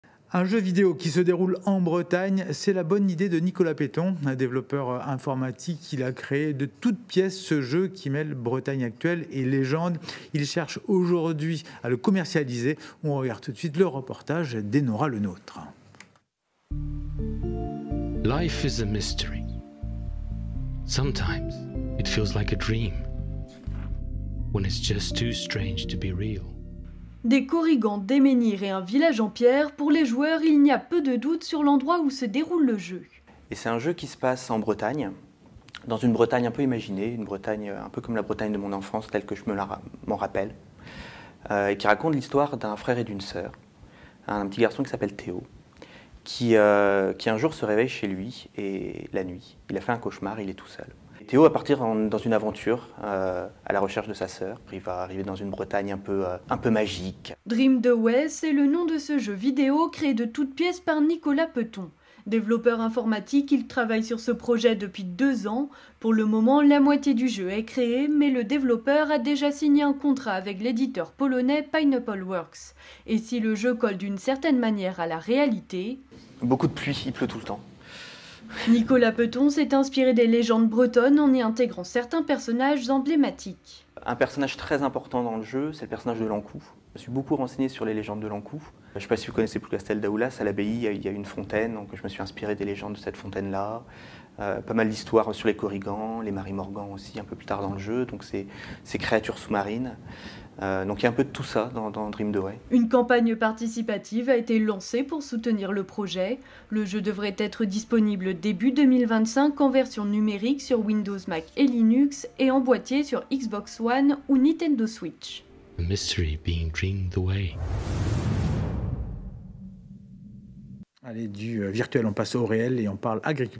Nicolas Petton about Dreamed Away (French TV channel Tebeo)
This short story was featured on TV the 02/19/24 on French TV channel Tebeo.